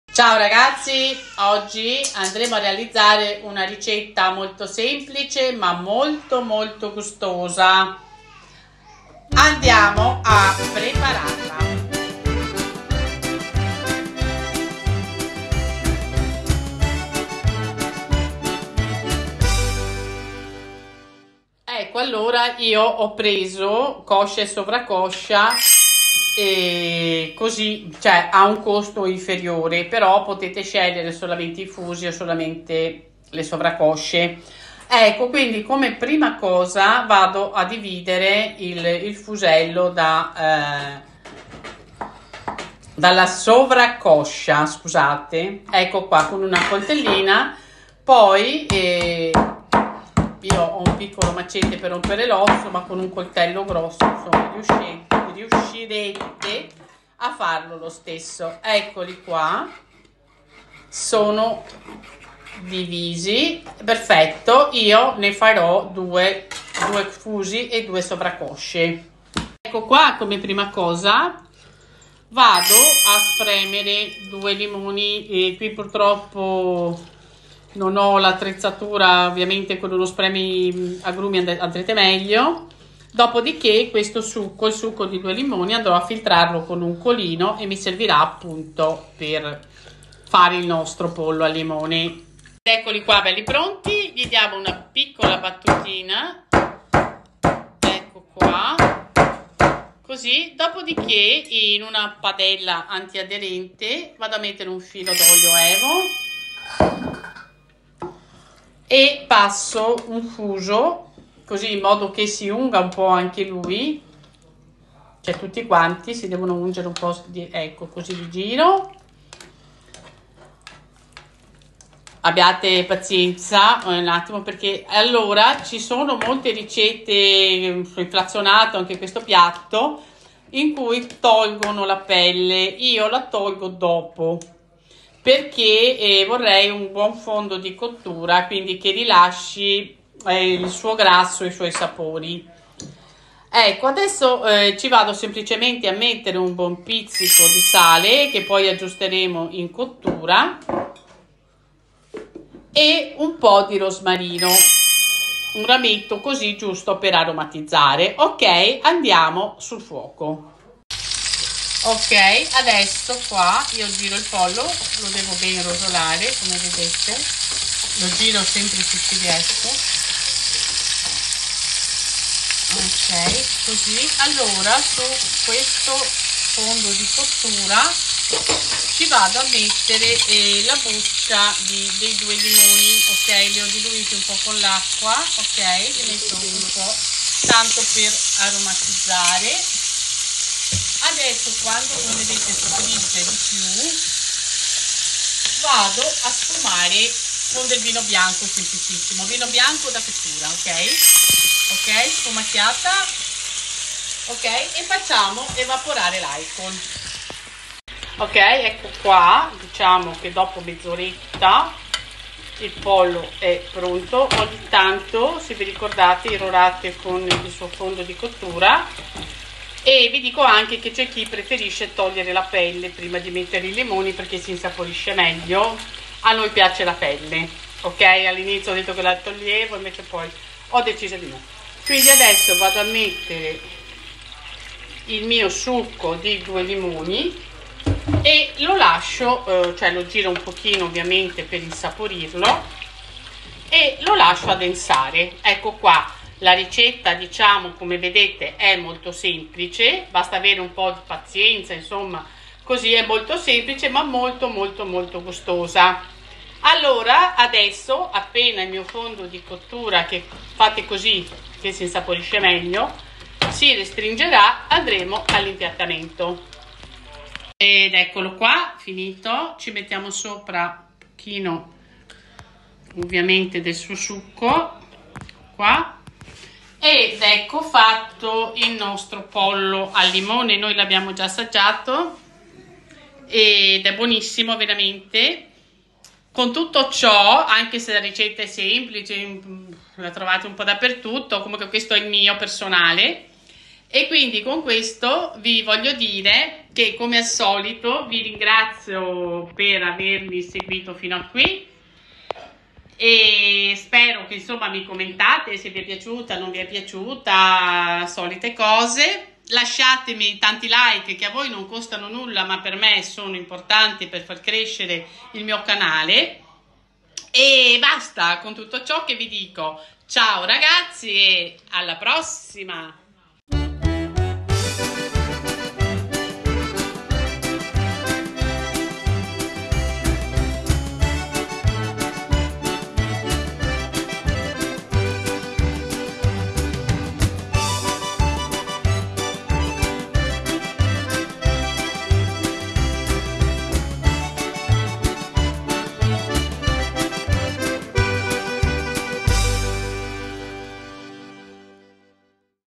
# Le Ricette di Patty Pollo al Limone
Cari Pattyni, Care Pattyne

REALIZZO UNA RICETTA MOLTO DIFFUSA MA A MODO MIO
“-Pollo al Limone-“
E’ DA PROVARE, VERAMENTE MOLTO BUONA
 ANCORA UNA VOLTA CHIEDO:
NELLA COMMUNITY SE VOLETE LASCIATE LE VOSTRE RICETTE E PROVERÒ AD REALIZZARE UNA A CASO